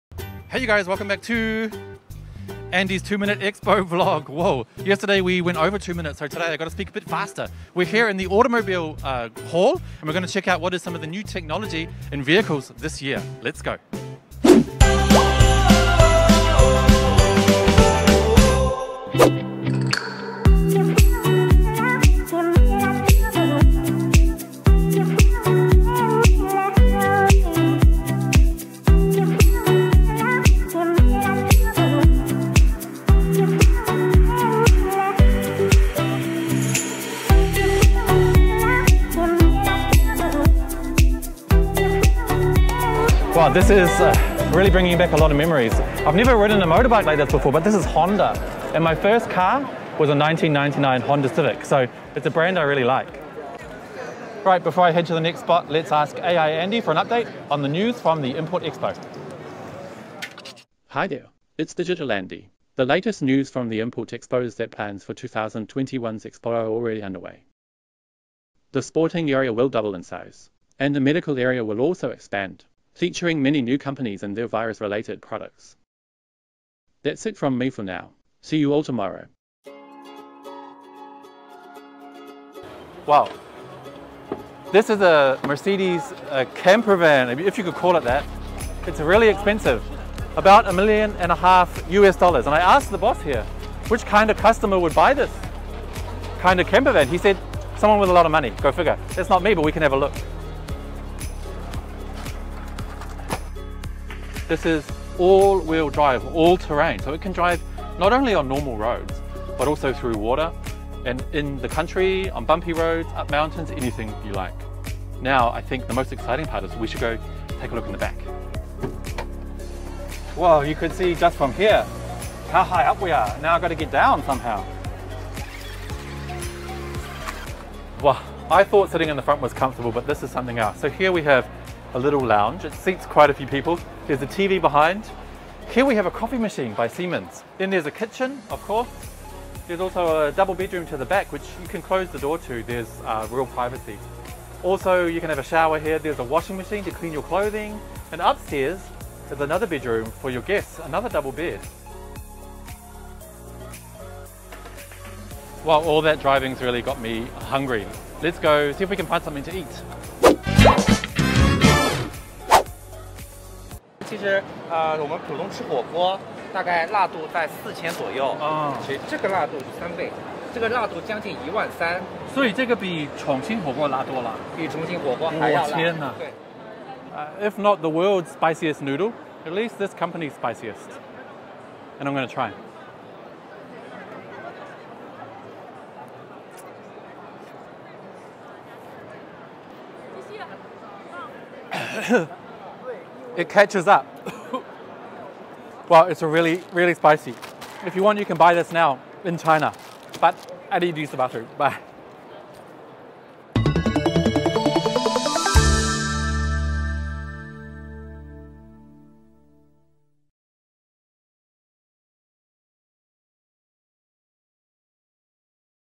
Andy's CIIE 2020 vlog #2:Million-dollar camper truck and world's spiciest noodles
It turned into a spicy day yesterday after checking out some of the world’s latest cars and trucks got me a bit hungry. My snack of choice would lead to tears, and some regrets.